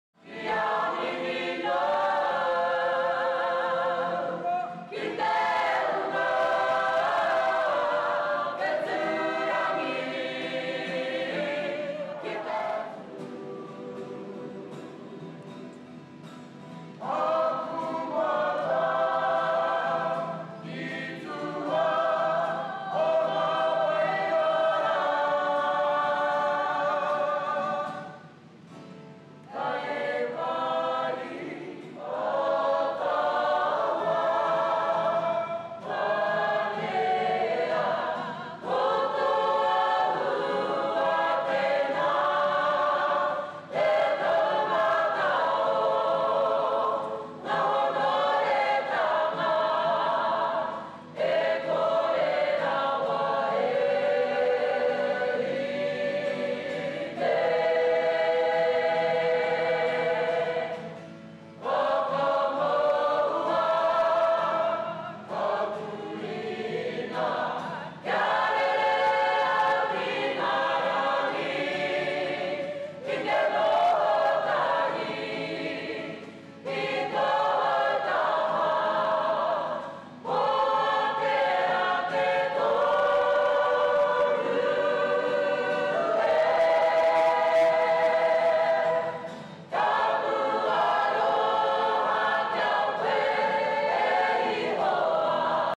ONLINE EXTRA: Manawa Mai Tawhiti
A short collection of songs from Manawa Mai Tawhiti.